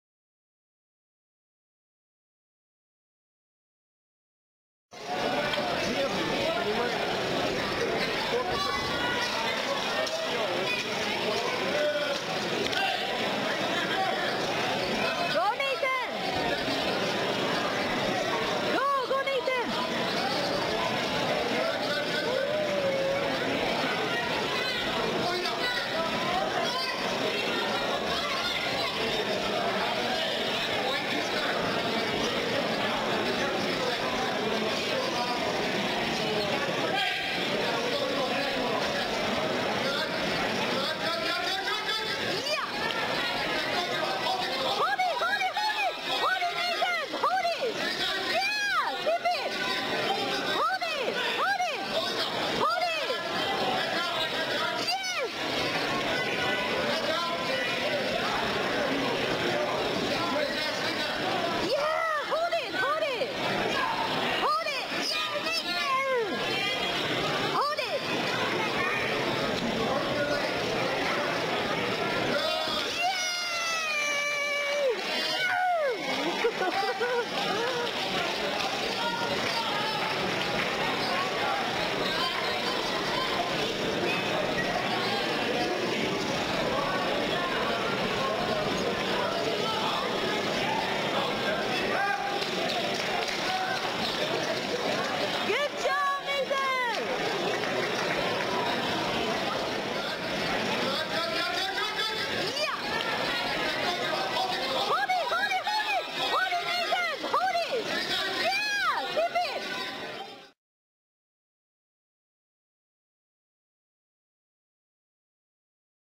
Nathan wins his 1st match in the Colorado State Championship with a hold down
Fort Collins Judo Club's Nathan wins his first match in the 2009 Colorado State Judo Championship.
He takes the initiative in the match attacking constantly and ends up pulling off a hold down. Final result: ippon.